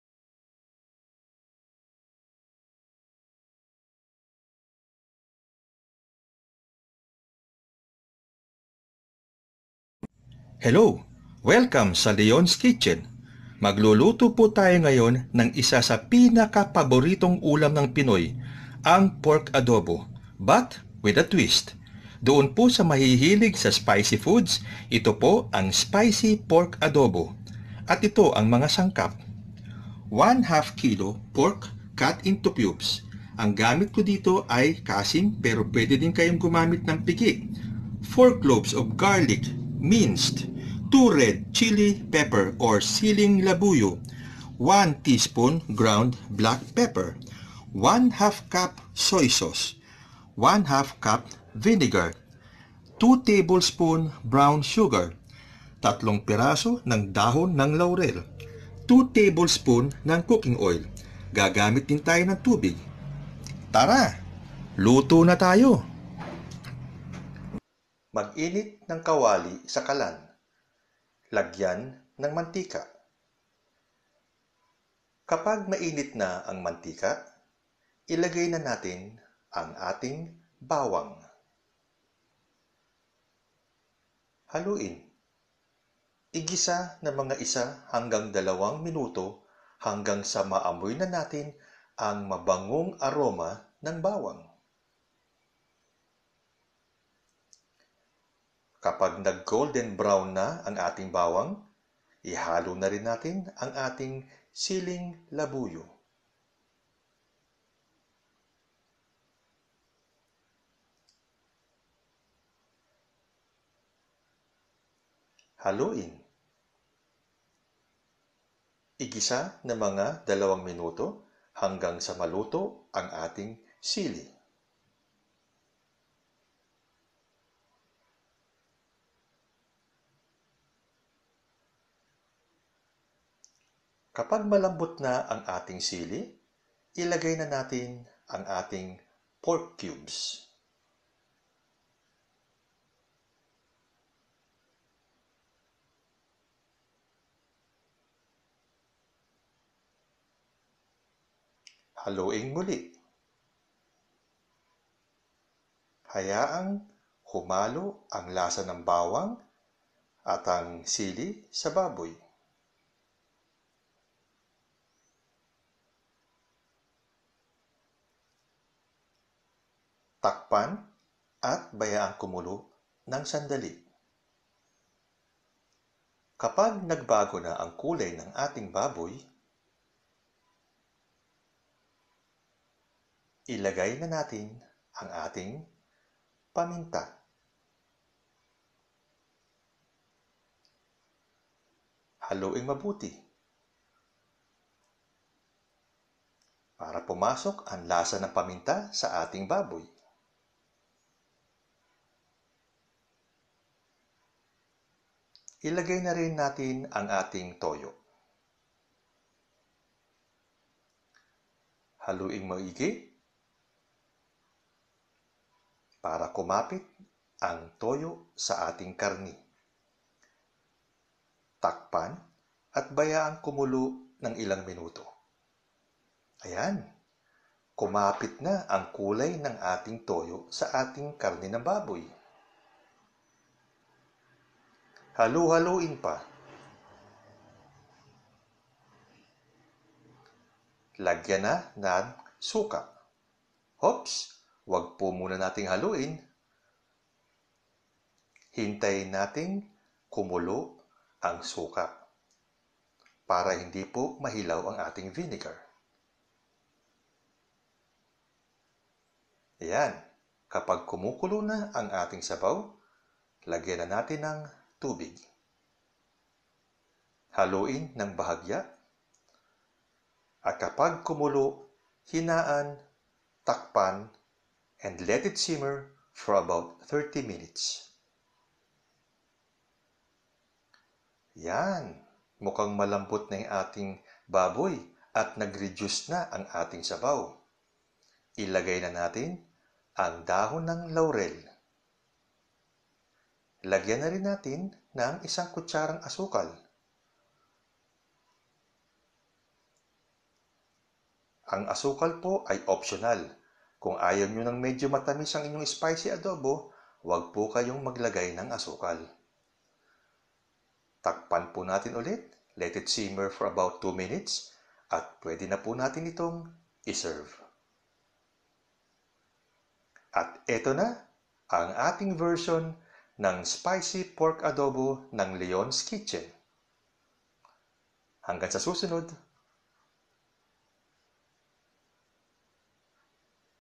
Spicy Pork Adobo
Leon's Kitchen

Ingredients
500 grams pork cubes
4 cloves garlic, minced
2 red chili pepper or siling labuyo
1 tsp ground black pepper
1/2 cup soy sauce
1/2 cup vinegar
2 tbsp brown sugar
3 pcs laurel leaves
2 tbsp cooking oil
1 cup water